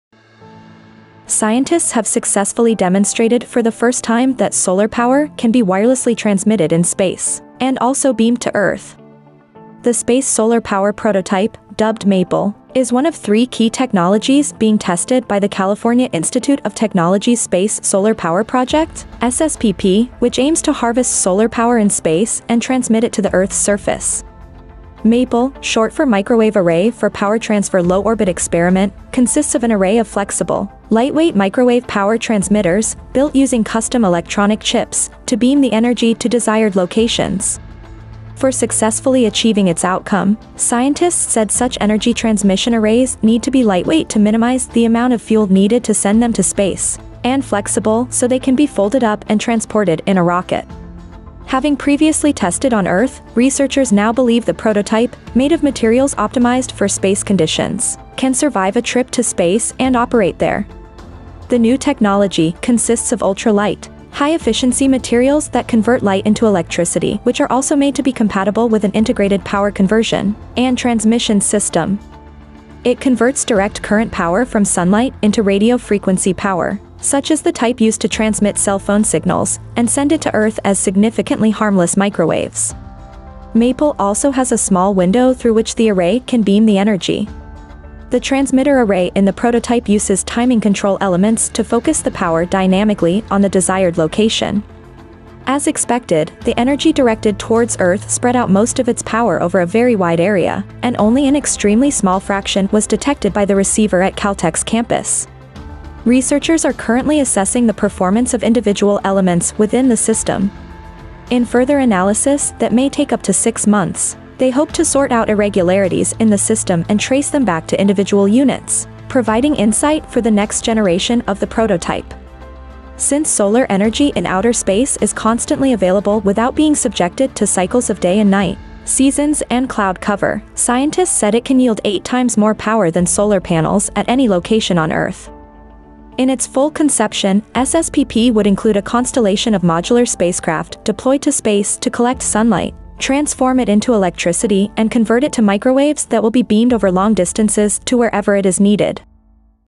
Scientists Beam Solar Power From Space to Earth
Researchers with the Space Solar Power Project at Caltech recently completed the first successful wireless power transfer using the Microwave Array for Power transfer Low orbit Experiment (MAPLE).